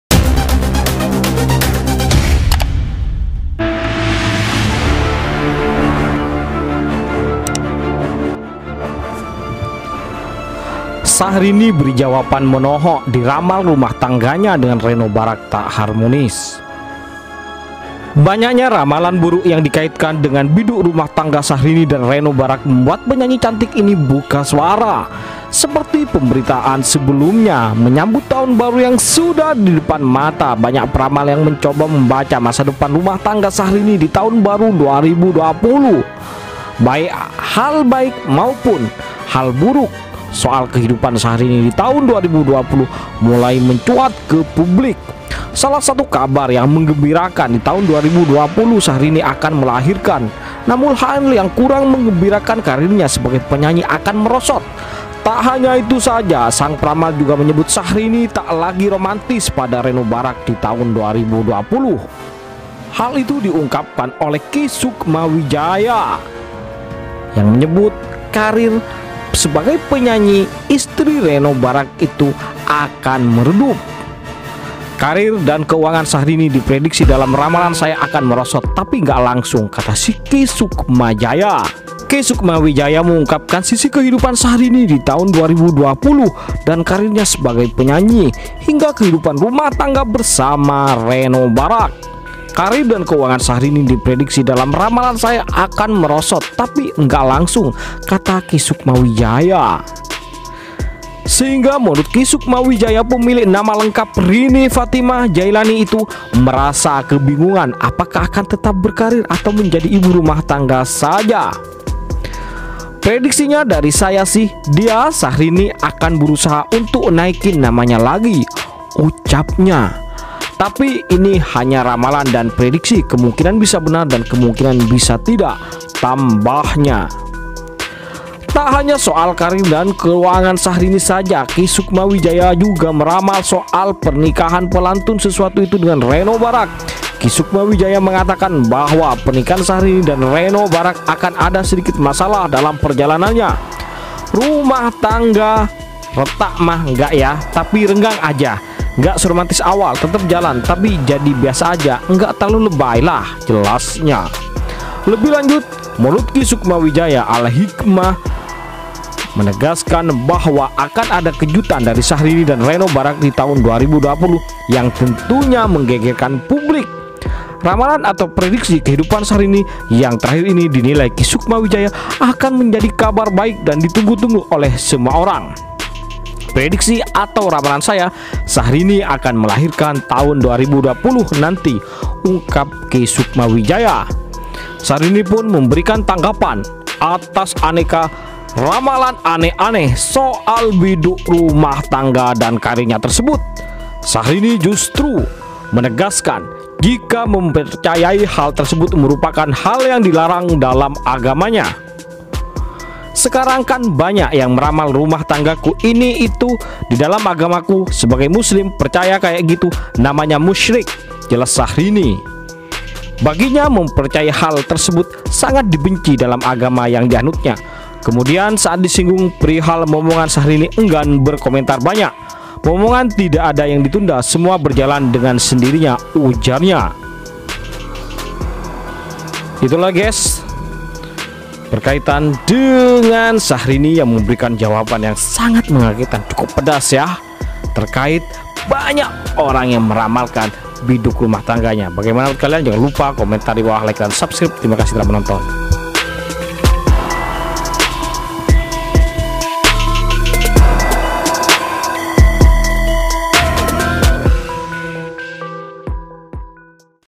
Akhirnya!! Syahrini Berikan Jwb MN0H0K,Atas RAMALAN Hubungannya dg Reino Barack Tak Harmonis,Cerai?
Akhirnya!! Syahrini Berikan Jawaban Menohok, Atas Ramalan Biduk Rumah Tangga dg Reino Barack yg Tak Harmonis,Cerai?

Jangan Lupa: _
Thankz!!..